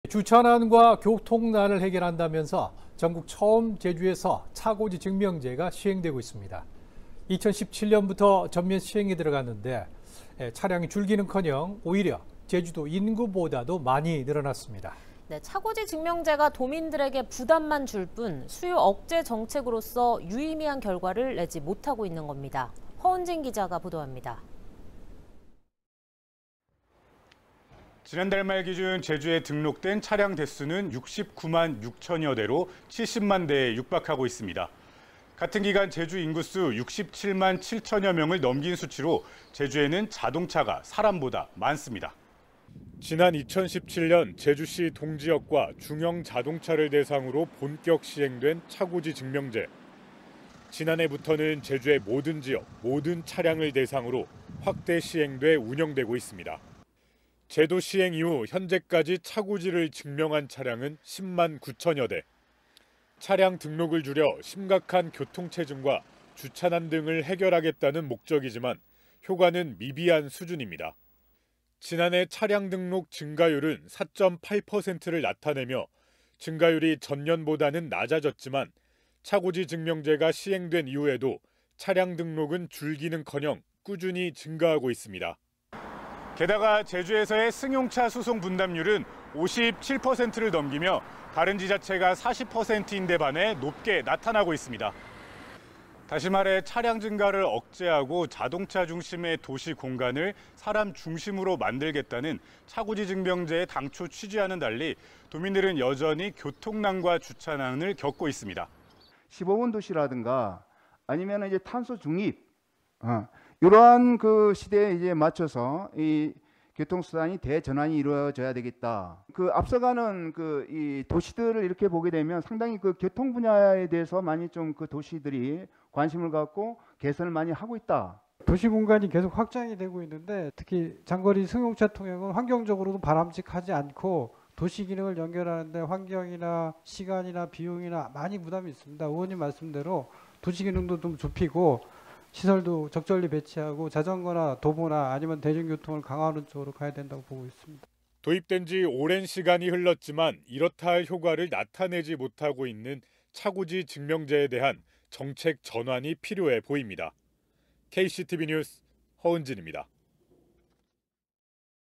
차고지증명제 효과 '글쎄'
주차난과 교통난 같은 문제를 해결하기 위해 제주에서는 차량 증가 억제를 위한 차고지 증명제가 시행되고 있습니다.

사실상 2017년부터 본격 시행되며 많은 시간이 흘렀는데 차량 등록이 줄기는 커녕 오히려 제주 인구 수를 넘겼습니다.

차고지증명제가 유의미한 결과를 내지 못하고 있는 겁니다.